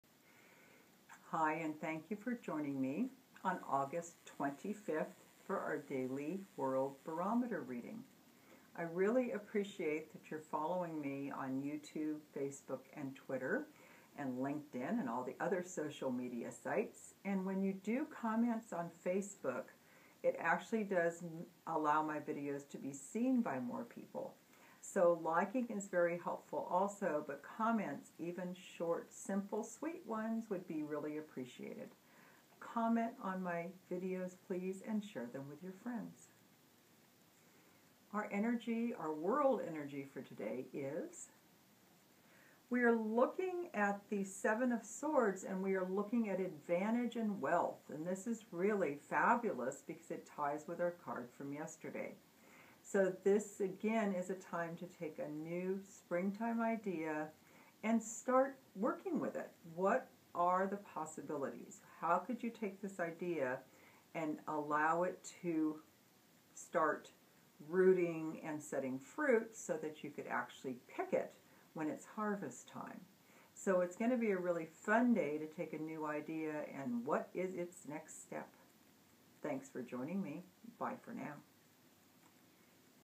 August 25 Today's World Barometer Tarot Card Reading is Seven of Swords
August 25 Today's World Barometer Tarot Card Reading is Seven of Swords.  Today is a great day to work on an idea that can bring more abundance to you.  What possibilities can you work on today?  Psychic reading of today's energy by Margaret VanLaanMartin, Psychic and Medium, ageofaquariuschico.com